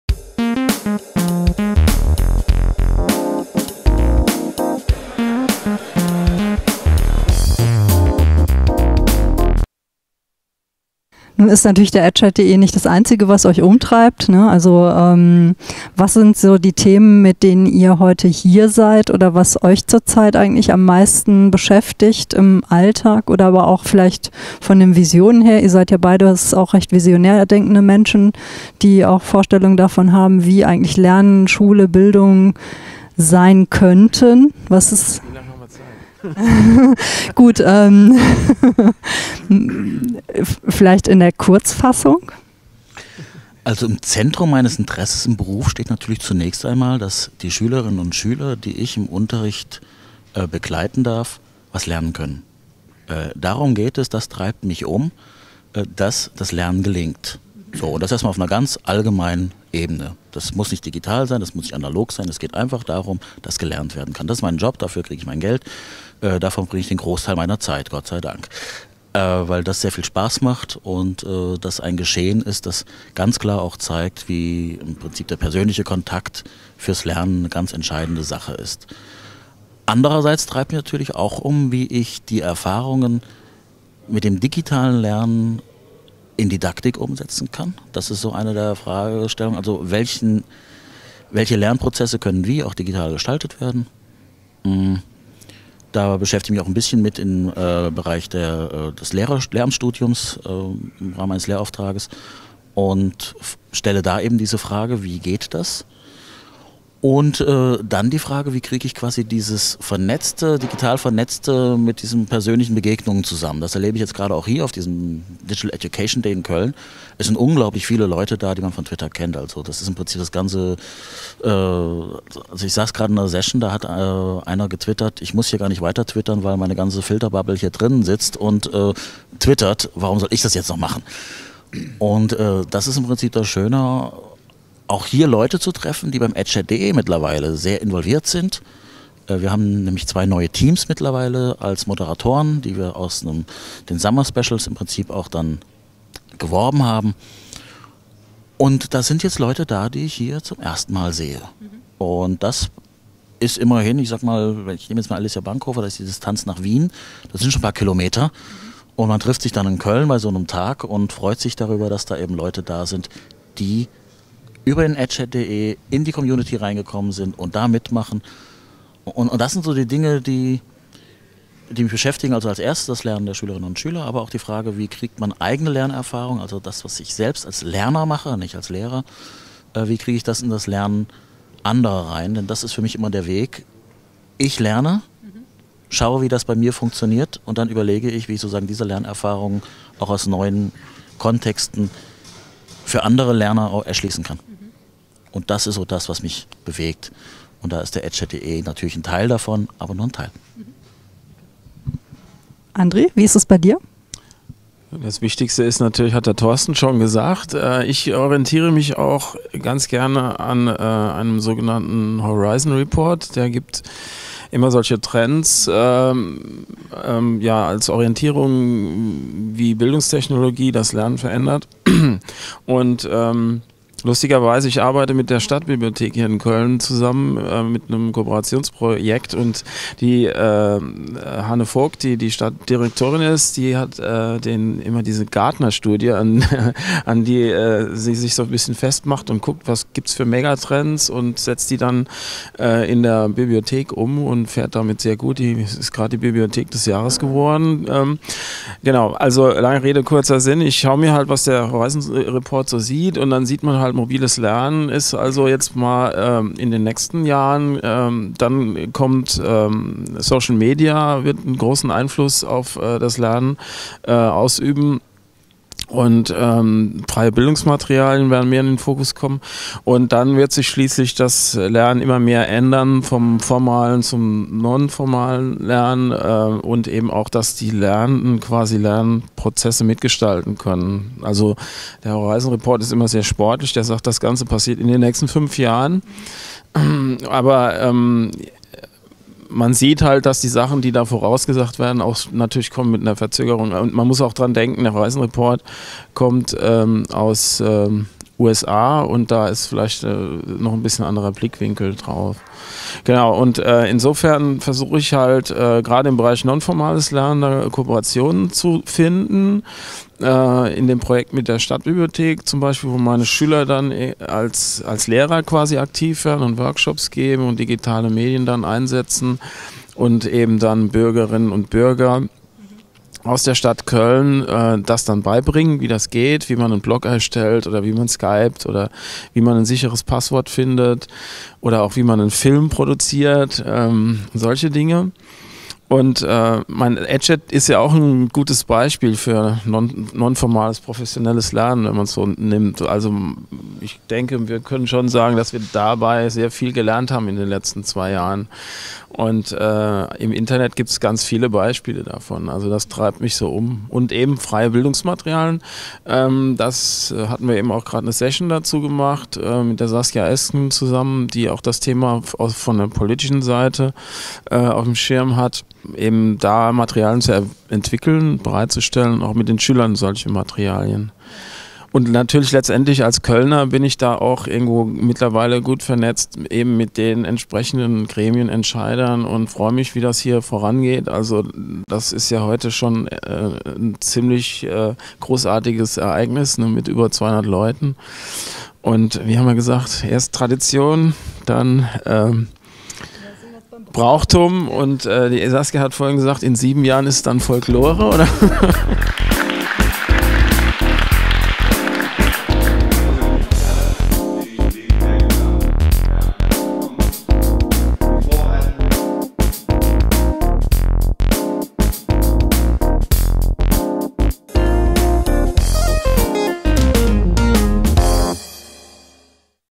DED15-Interview EdchatDE 02: Lernen in Digitalien
Am Rande des Digital Education Day in Köln, auf dem Bildungscamp am 24. Oktober 2015, nutzte Moderatorin Wibke Ladwig die Gelegenheit, die beiden EdchatDE-Gründer Torsten Larbig und André Spang zu interviewen. Hier der zweite Part der vier Teile.